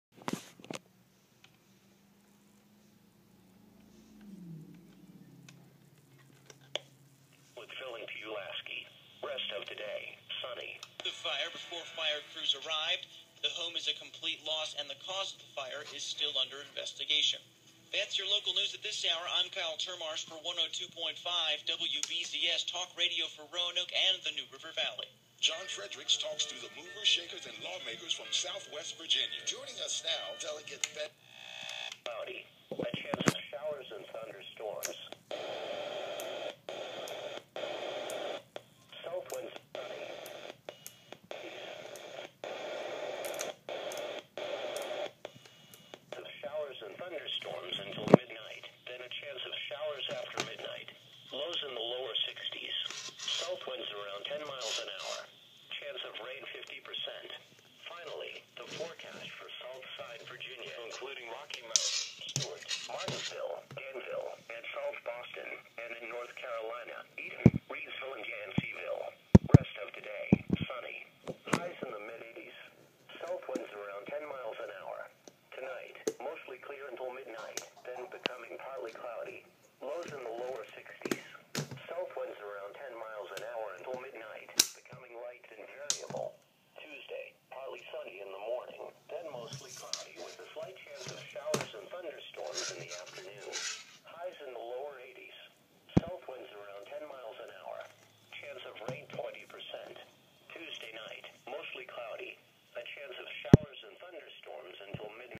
My New Midland Weather and fm and am radio
I found this at a local goodwill for 59 cents and the only things wrong with it is the batteries that came with it was bad and it needed to be cranked and one of the strap holders is broke which im going to try and fix but other then that its works great and on midland website there 60 bucks